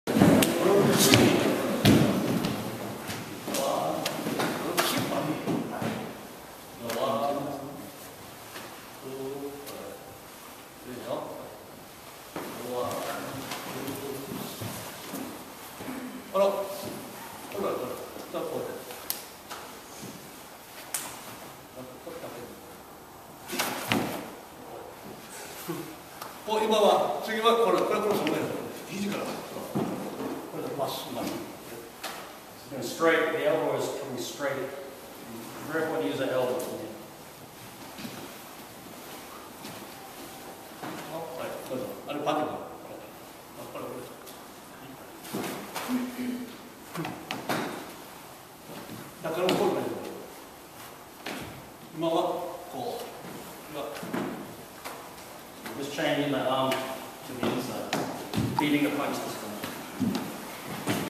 Taira Sensei Geki Sai Dai Ni Ending sequence Inside
Masaji Taira Sensei 2012 in Napanee, On at the Napanee Karate Club under the direction of Kyoshi Brian Lowry showing the bunkai for the ending of Geki Sai Dai Ni - inside version.  Uke Kyoshi Paul Enfield.